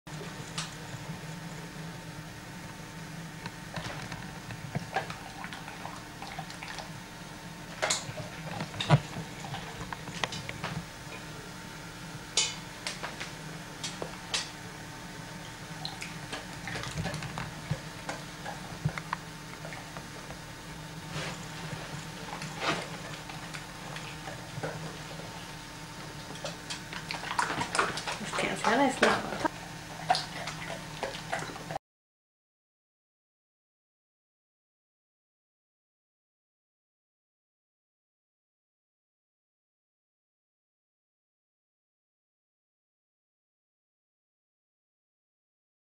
Parrot Feeds Malamute Spaghetti
This parrot and Alaskan malamute are quite the team. The parrot hops onto the stovetop and uses its beak to snag some spaghetti, which it then drops into dog's gaping mouth below.